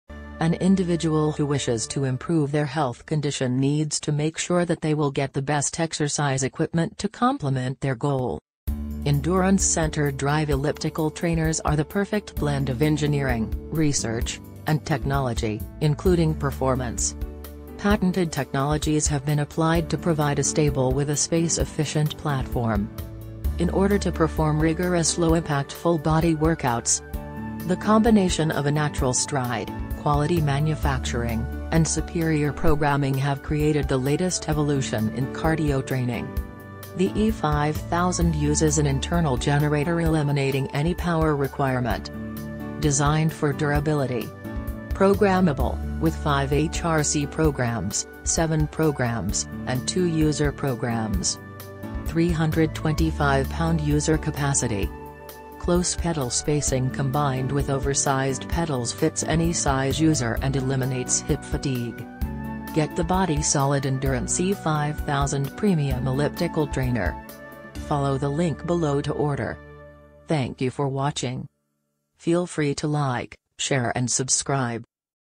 body solid endurance E5000
Body-Solid Endurance E5000 Premium Elliptical Trainer

An individual who wishes to improve their health condition needs to make sure that they will get the best exercise equipment to complement their goal.

Endurance Center Drive elliptical trainers are the perfect blend of engineering, research, and technology, including performance.
Patented technologies have been applied to provide a stable with a space-efficient platform.
In order to perform rigorous low impact full body workouts.
The combination of a natural stride, quality manufacturing, and superior programming have created the latest evolution in cardio training.
The E5000 uses an internal generator eliminating any power requirement.
Designed for durability.

Programmable, with five HRC programs, seven programs, and two user programs.
325-pound user capacity.
Close pedal spacing combined with oversized pedals fits any size user and eliminates hip fatigue.

Get the Body-Solid Endurance E5000 Premium Elliptical Trainer